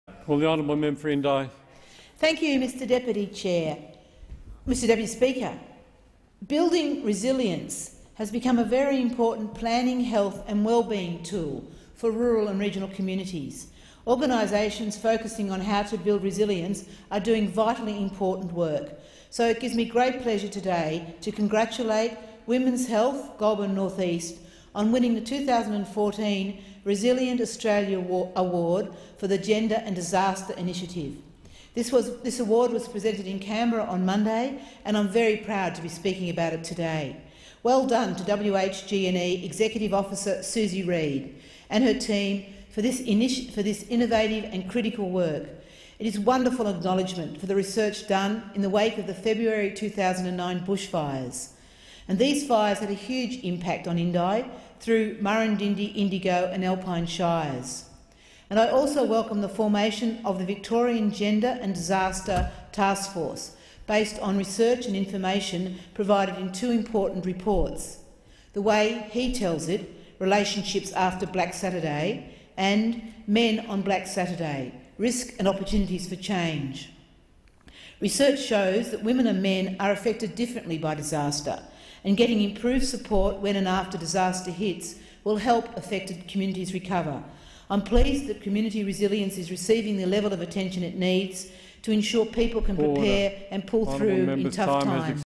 Women's Health Goulburn North East wins 2014 National Resilience Award
Independent Member for Indi, Cathy McGowan AO, congratulates Women's Health Goulburn North East on winning the 2014 National Resilience for the Gender & Disaster initiative